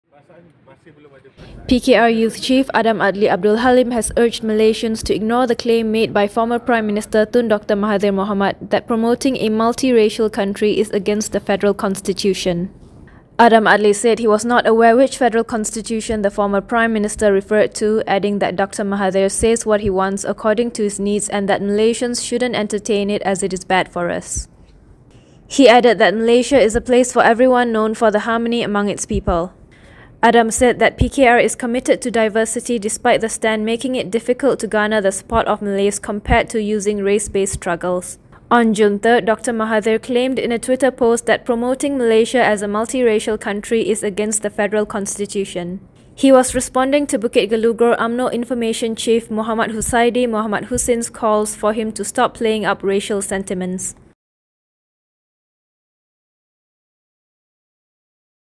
Adam Adli: Ignore Dr M
PKR Youth chief Adam Adli Abdul Halim has urged Malaysians to ignore the claim made by former prime minister Tun Dr Mahathir Mohamad that promoting a multiracial country is against the Federal Constitution.

Adam Adli said he was not aware which Federal Constitution the former prime minister referred to, adding that Dr Mahathir says what he wants according to his needs and that Malaysians shouldn't entertain it, as it is bad for us.

He added that Malaysia is a place for everyone, known for the harmony among its people.

Adam said that PKR is committed to diversity despite the stand making it difficult to garner the support of Malays compared to using race-based struggles.

On June 3, Dr Mahathir claimed in a Twitter post that promoting Malaysia as a multiracial country is against the Federal Constitution.

He was responding to Bukit Gelugor Umno information chief Mohd Huzaidi Mohd Hussin's calls for him to stop playing up racial sentiments.